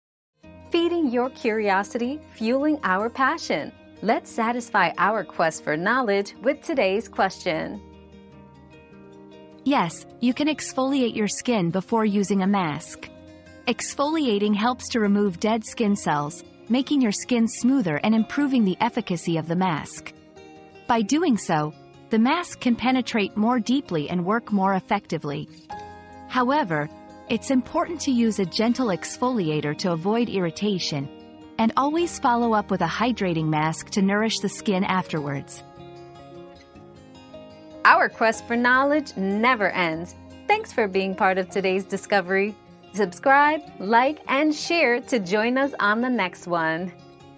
Can we exfoliate our skin before using a mask?
Maximizing Your Mask: Exfoliating Before Application 👉 Skin Prep Tip 👉 Learn why exfoliating before applying a mask can improve its effectiveness by allowing it to penetrate deeper into the skin. Remember to choose a gentle exfoliator and follow up with a hydrating mask for best results.

Laura S. Harris (2024, April 27.) Can we exfoliate our skin before using a mask?
    🌐 AskAbout.video